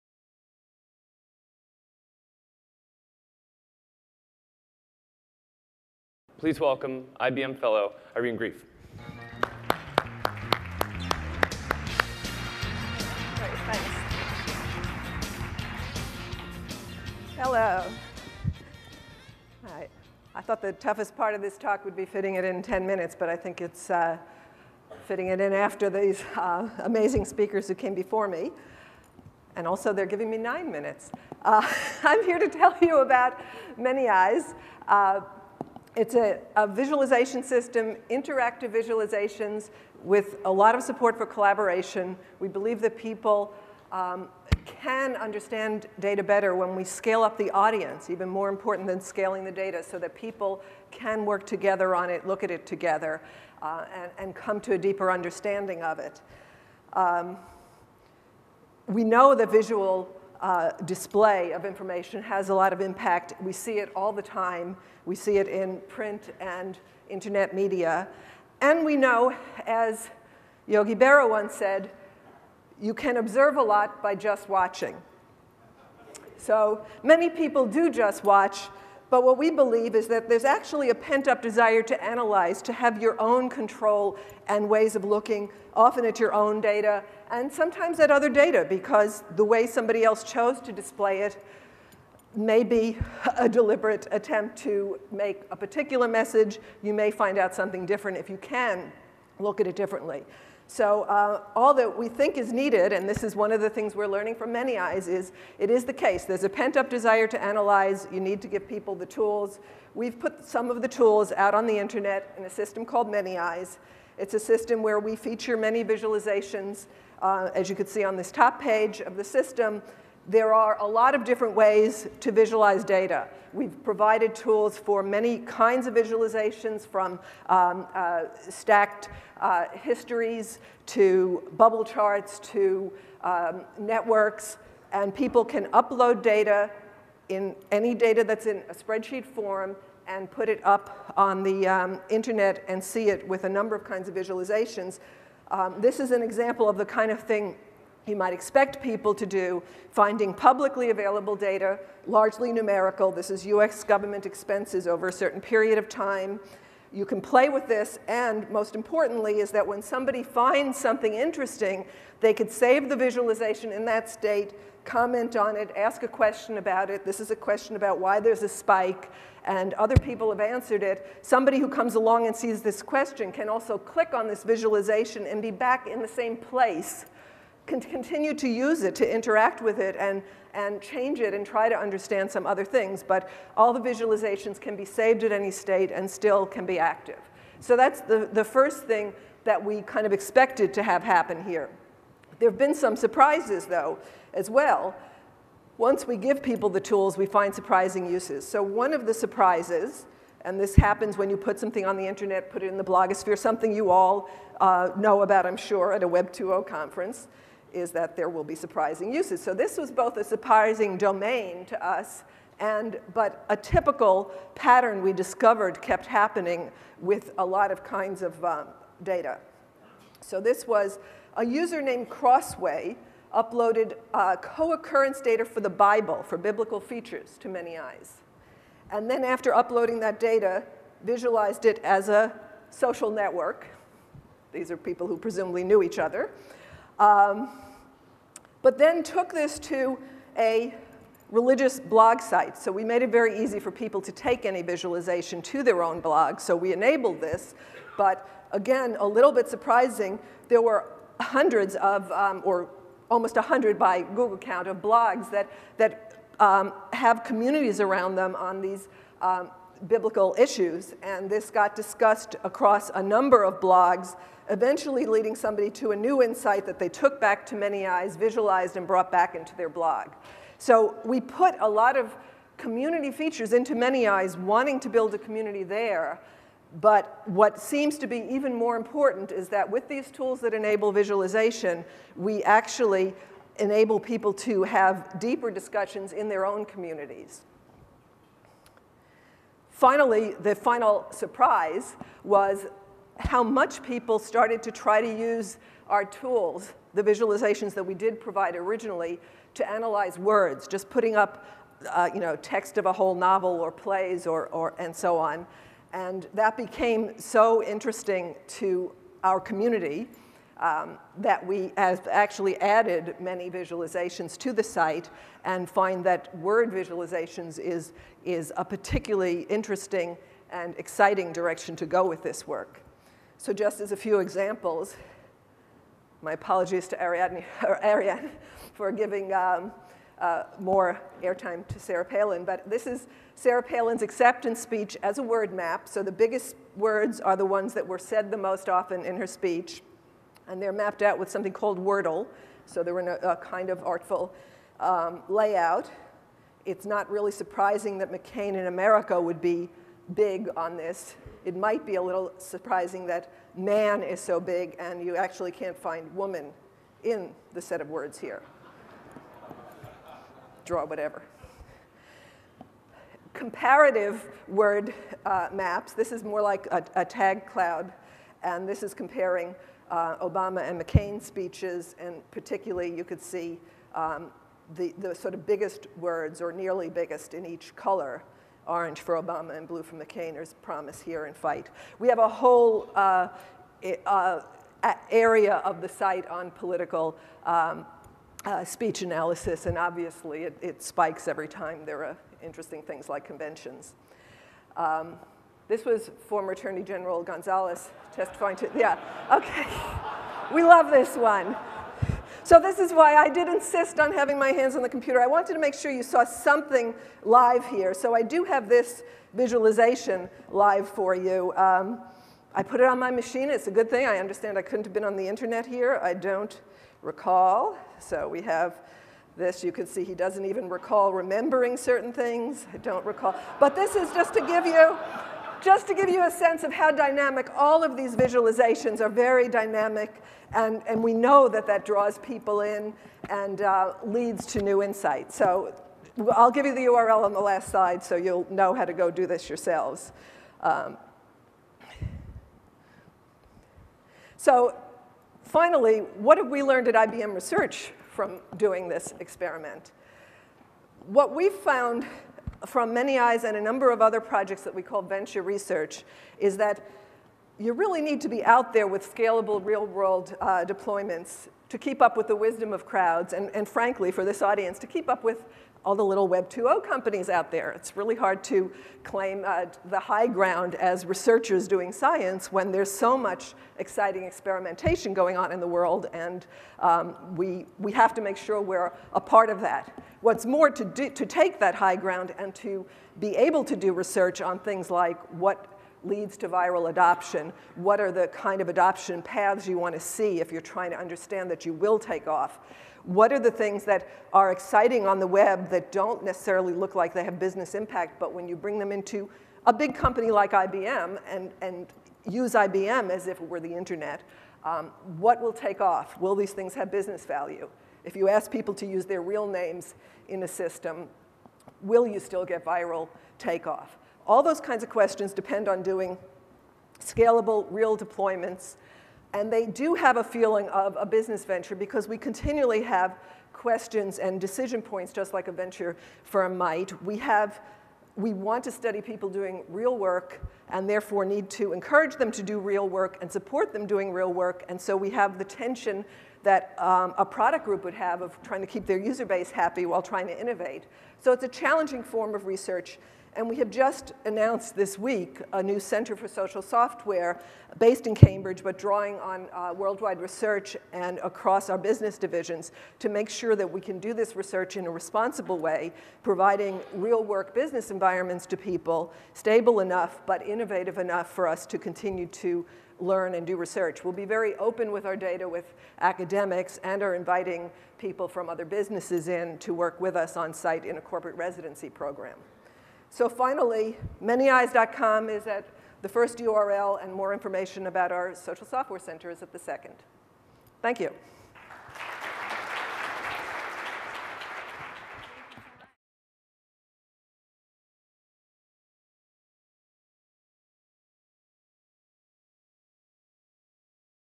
Web 2.0 Expo NY: Irene Greif (IBM), What ManyEyes Knows
ManyEyes is a data collaboration and visualization service. Its been used by communities across the world to decipher what their data is telling them. Irene shares some of their stories.